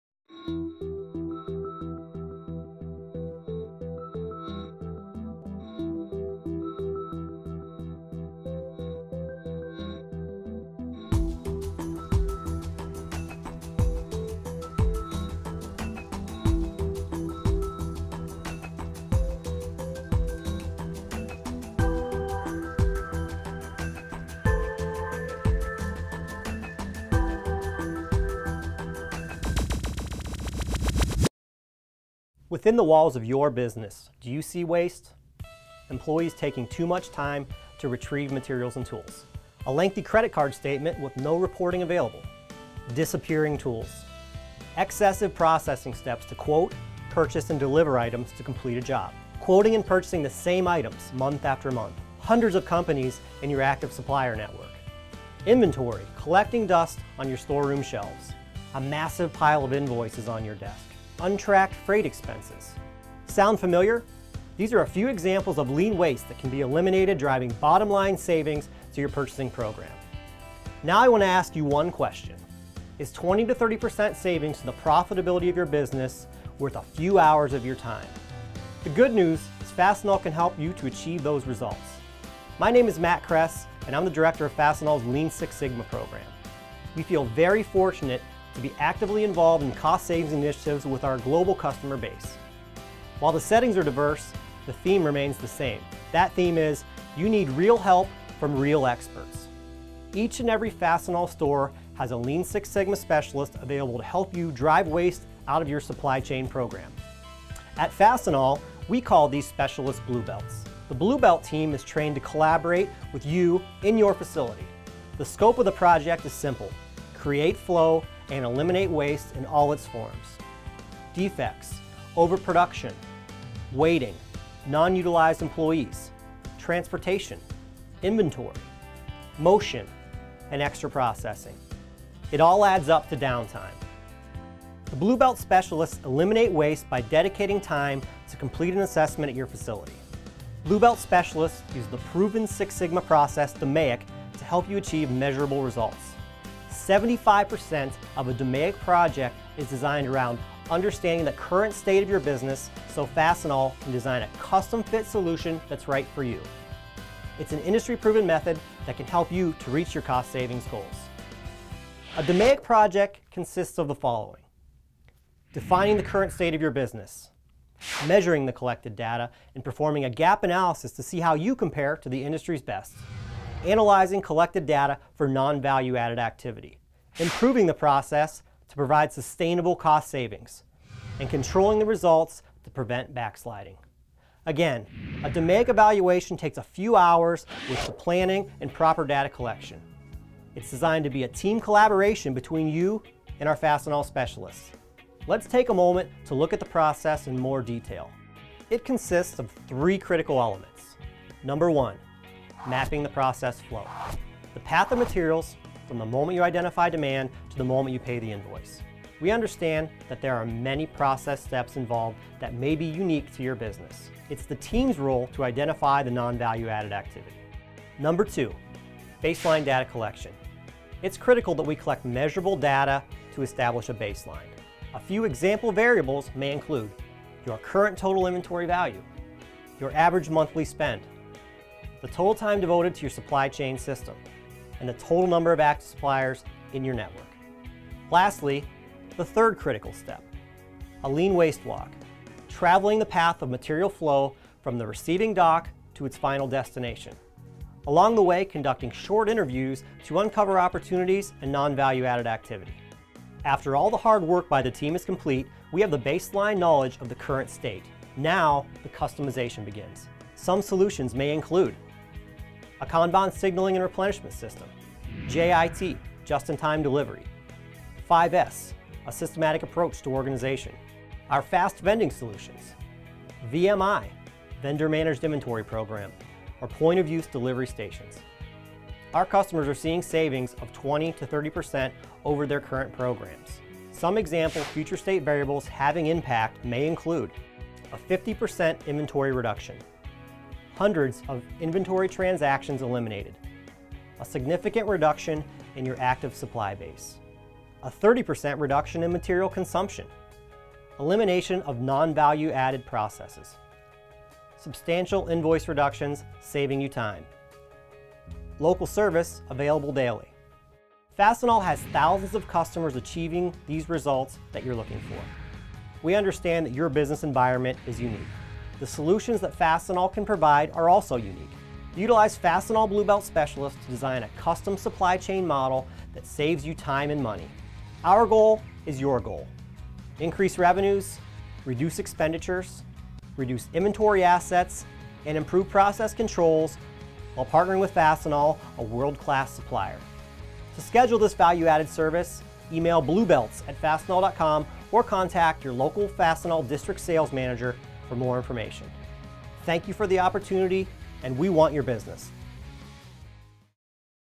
Fastenal Lean Solutions
Fastenal's team of Lean Six Sigma Black Belts and Sensei can work with your personnel to map how product moves through your current system and identify bottlenecks, extra steps, and other sources of waste. You'll be presented with new opportunities to create flow, eliminate waste, and drive out costs.